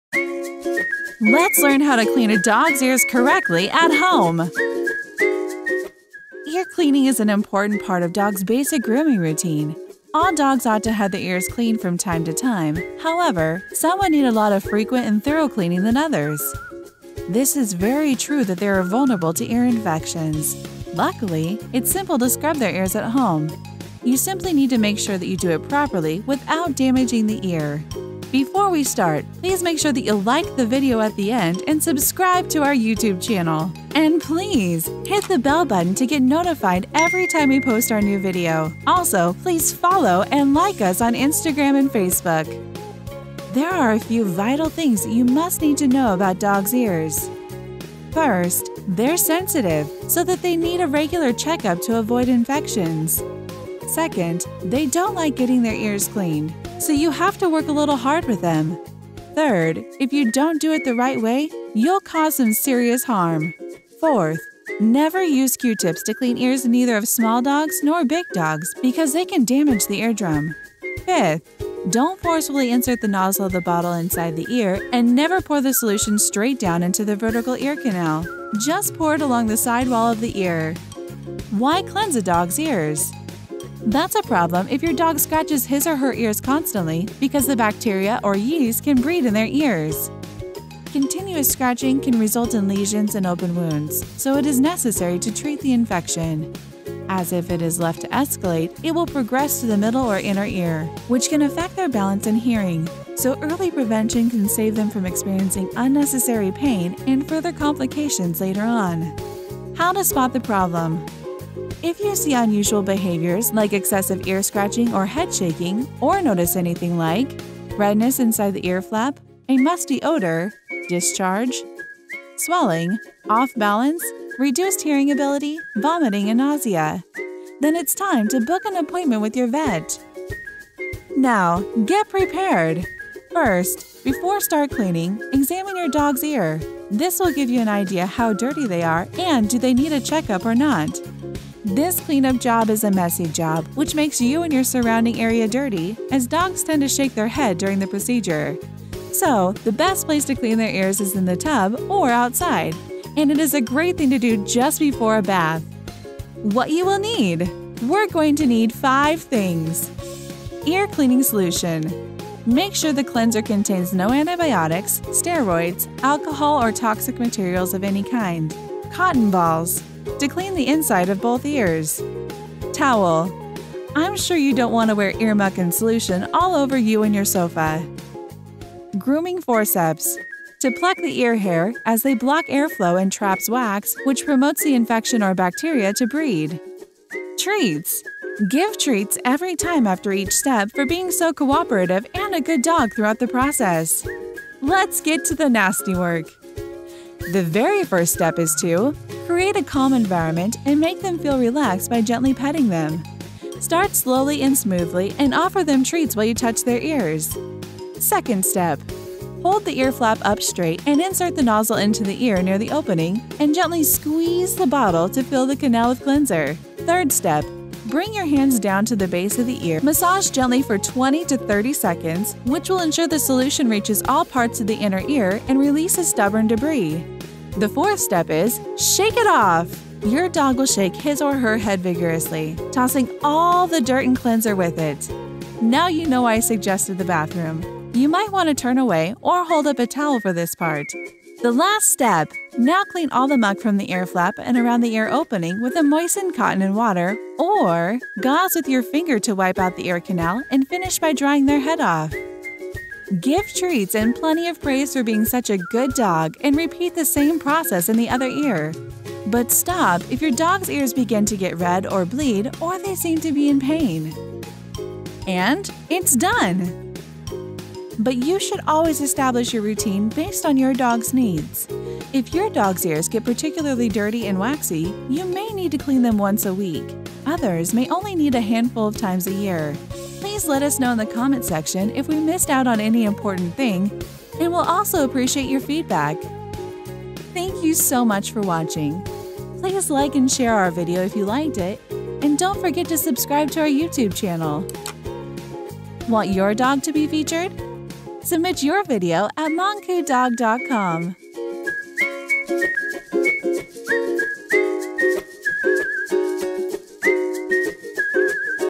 How to Clean Your Dog's Ear in 5 Simple Steps || Avoid ear infection in dogs ll
Every human clean their ears on time and by themselves but dogs can't clean their ears by themselves. doggrooming is one of the essential practice for their healthy life. As they have every sensitive ears, Today Monkoodog will should know howtocleanyourdogsear correctly without damaging their ear step-by-step in five easy steps at home like professionals.

US people buy Ear Cleaning Solution

This is very true that they are vulnerable to ear infections. Luckily, it's simple to scrub their ears at home.There are a few vital things that you must need to know about dogs’ ears:

First. They’re sensitive so that they need a regular checkup to avoid infections.
Second. They don’t like getting their ears cleaned, so you have to work little hard with them.
Third. If you don’t do it the right way, you’ll cause some serious harm.
Fourth. Never use Q-tips to clean the ears neither of small dogs nor big dogs because they can damage the eardrum.
Fifth. Don’t forcefully insert the nozzle of the bottle inside the ear and never pour the solution straight down in the vertical ear canal. Just pour it along the sidewall of the ear.

This question must have come into your mind many times. So today, i'll answer you question - Why cleanse a dog’s ear?

That’s a problem if your dog scratches his or her ears constantly because the bacteria or yeast has bred in their ears. Continuous scratching can result in lesions and open wounds. So, It is necessary to treat the infection as if it is left to escalate, it will progress to the middle or inner ear, which can affect their balance and hearing. So, early prevention can save them from experiencing unnecessary pain and further complications later on

How to spot the problem

If you see an unusual behavior like Excessive ear scratching or head shaking or notice anything like
1. Redness inside the ear flap
2. A musty odor
3. Discharge
4. Swelling
5. Off balance
6. Reduced hearing ability
7. Vomiting and nausea

Now
Get prepared.

First, before start cleaning, examine your dog’s ear. This will give you an idea of how dirty they are and do they need a cleanup or not.
This cleanup job is a messy job which makes you and your surrounding area dirty as dogs tend to shake their head during the procedure. So, The best place to clean their ears is in the tub or outside and it is a great thing to do just before a bath.

What all you need?
We’re going to need 5 things:
Ear cleaning solution: Make sure the cleanser contains no antibiotics, steroids, alcohol, or toxic materials of any kind, as they can cause irritation and should be at room temperature as dogs don’t like it cool.

Cotton balls: To clean the inside of both ears.

Towel: I’m sure you don’t want ear muck and solution all over on you or your sofa.

Grooming forceps:  To pluck the ear hair as they block the air flow and traps wax which promotes the infection or bacteria to breed.
Treats: Give treats every time after each step for being so cooperative and a good dog throughout the process.

Let’s get to the nasty work
The first thing to do is keep all the things within your reach because I’m sure you don't wanna start over the whole process from the halfway.

The very first step is to Create a calm environment and make them feel relaxed by gently petting them. Start slowly ‘n’ smoothly and offer them treats while you touch their ears.

Second step.
Hold the ear flap up straight and insert the nozzle into the ear near the opening and gently squeeze the bottle to fill the canal with cleanser.

Third step.
Bring your hands down the base of the ear which is the bottom part of the ear where cartilage can be felt. Massage gently for 20-30 seconds which will ensure the solution reaches all parts of the inner ear and releases stubborn debris.

The fourth step is ‘Shake it off!’
Your dog will shake his/her head vigorously, tossing all the dirt and cleanser with it. You might turn away or hold up a towel for this part. Now you know why I suggested the bathroom.

The Last step
Now clean all the muck from the ear flap and around the ear opening with a moistened cotton in water or gauze with your finger to wipe out the ear canal and finish by drying their head off.

Give treats and plenty of praise: But stop if your dog's ear begins to get red or bleed or they seem to be in pain.
And it’ done.

xxx